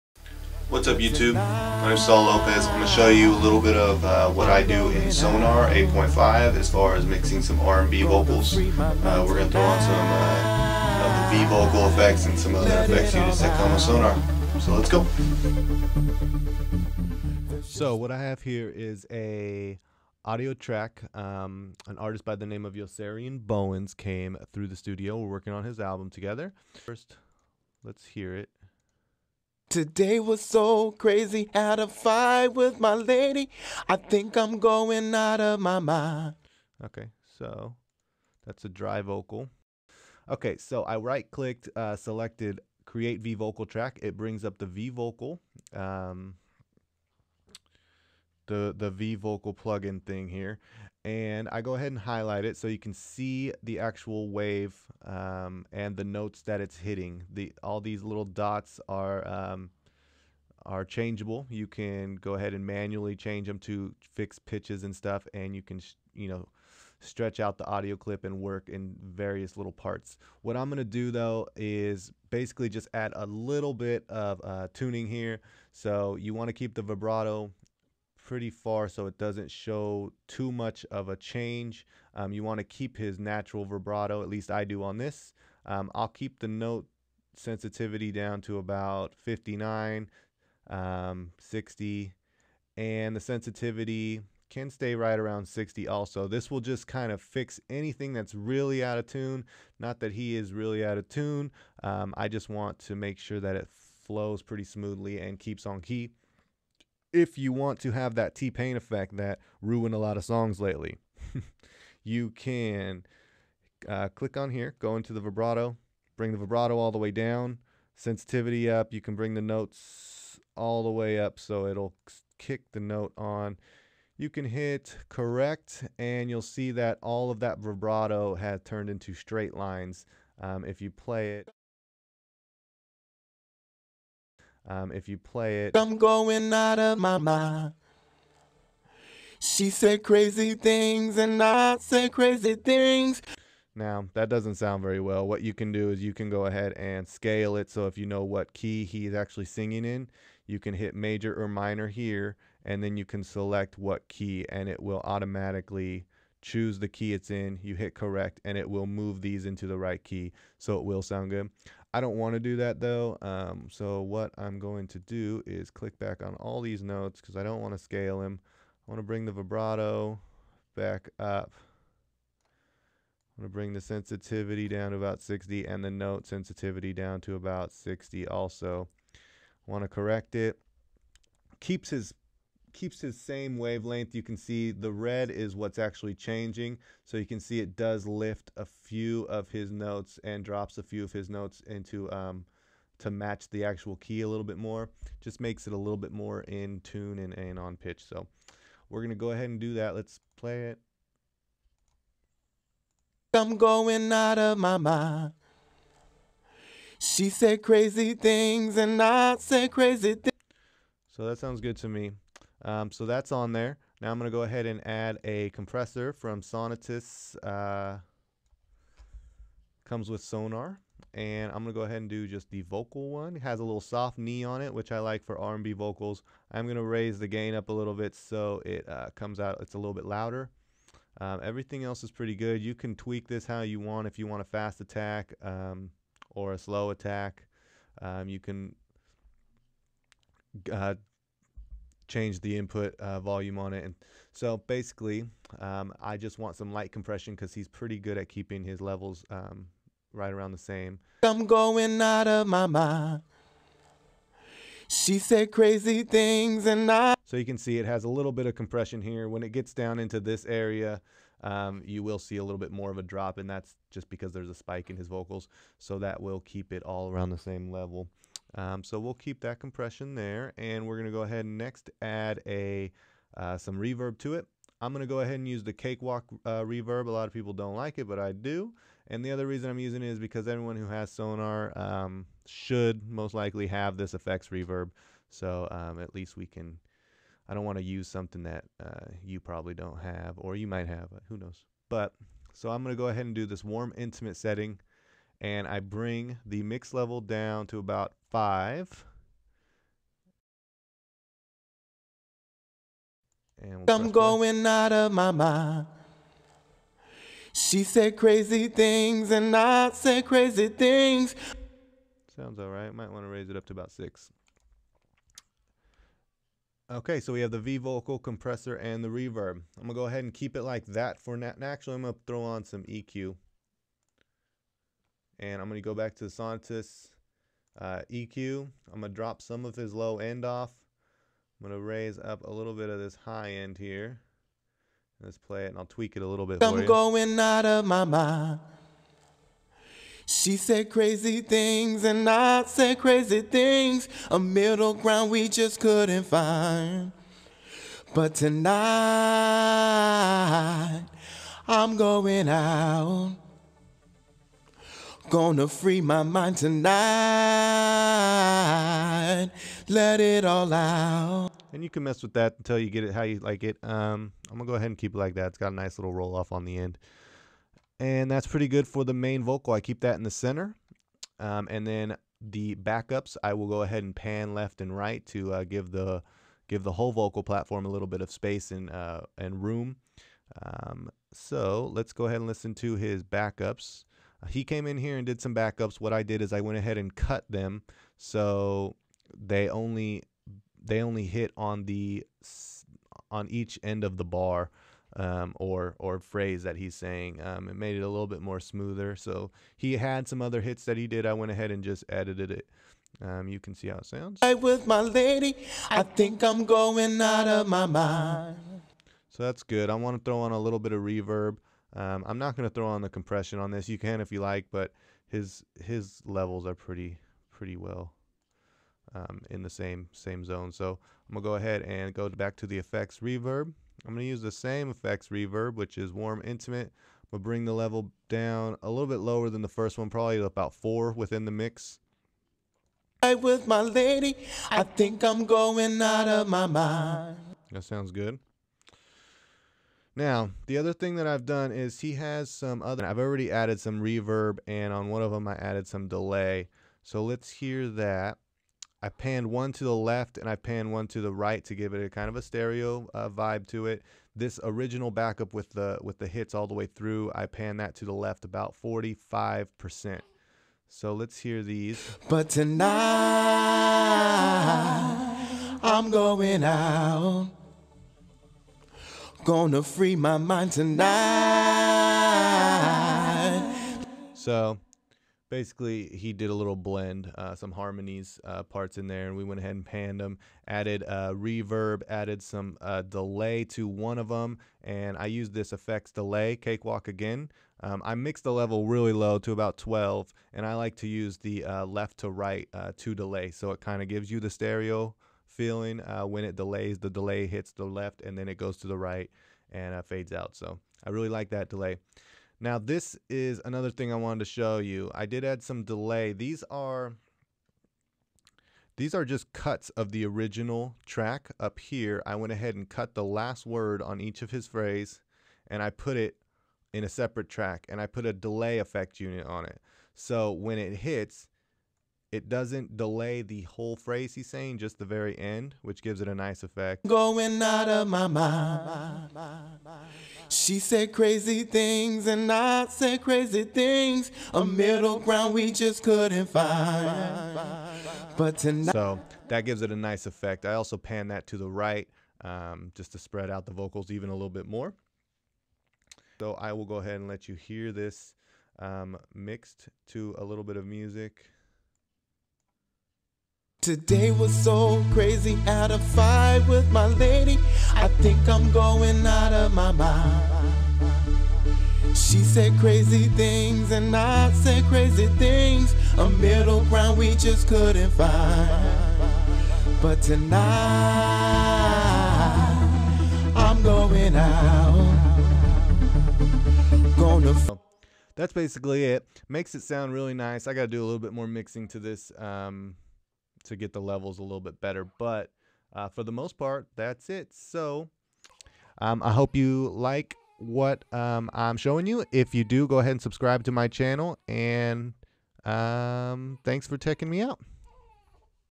mixing r&b Vocals in Sonar
Some fx units I use when mixing vocals in Sonar. Thanks